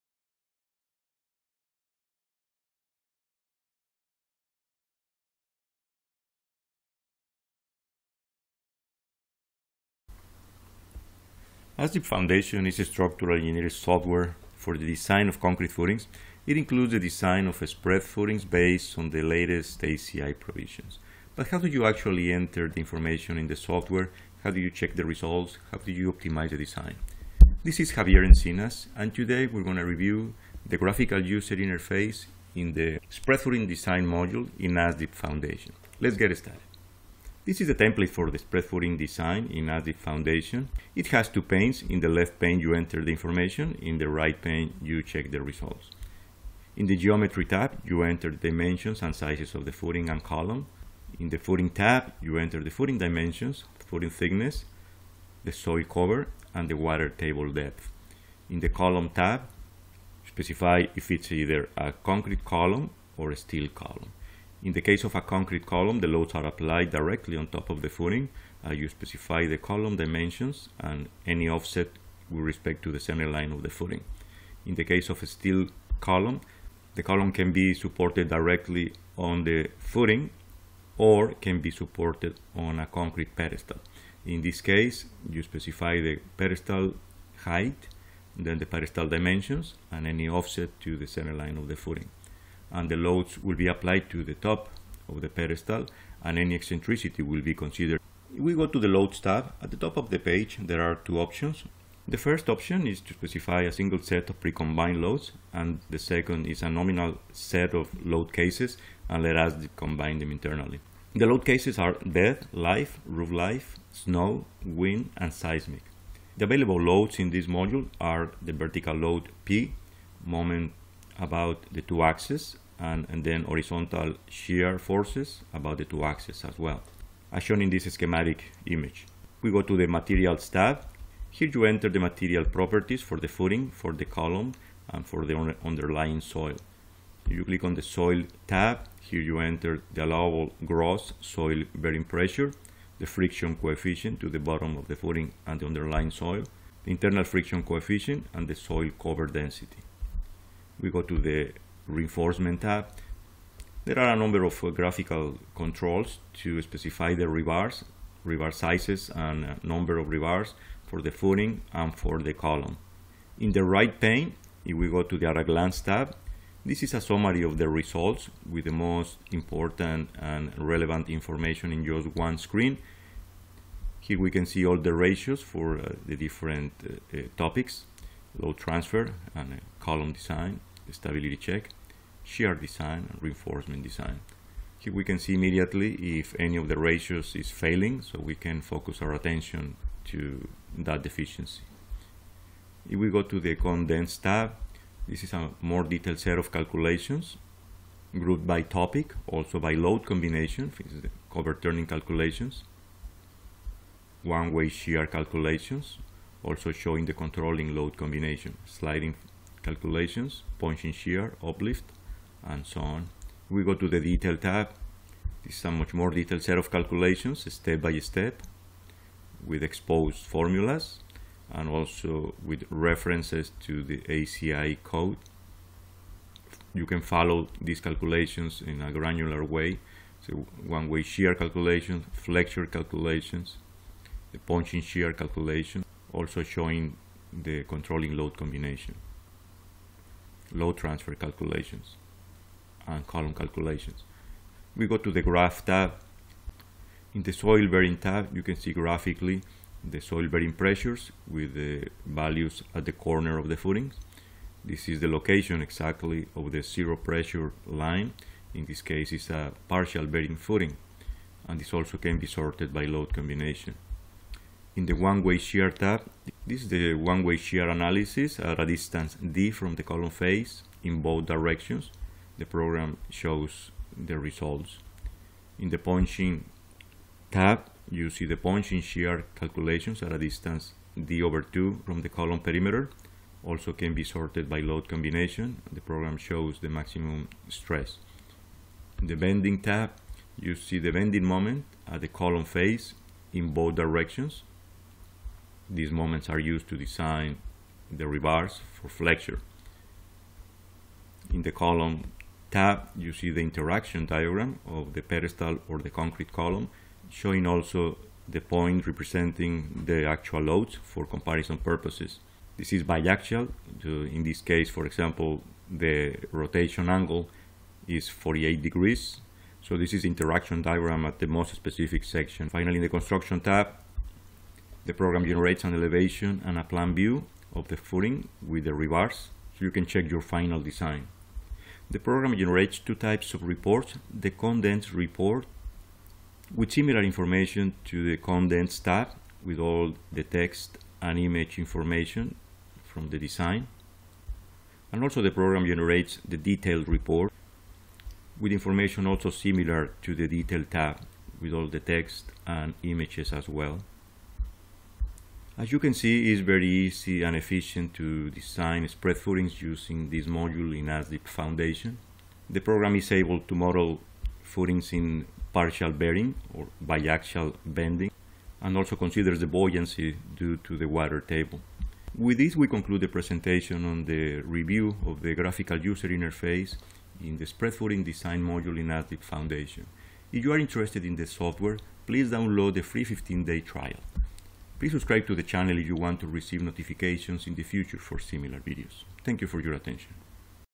Spread Footing Design Using ASDIP FOUNDATION
ASDIP FOUNDATION is a structural engineering software for design of concrete footings. It includes the design of spread footings based on the latest ACI provisions. This is an overview of the graphical user interface and the required steps for spread footing design using ASDIP FOUNDATION.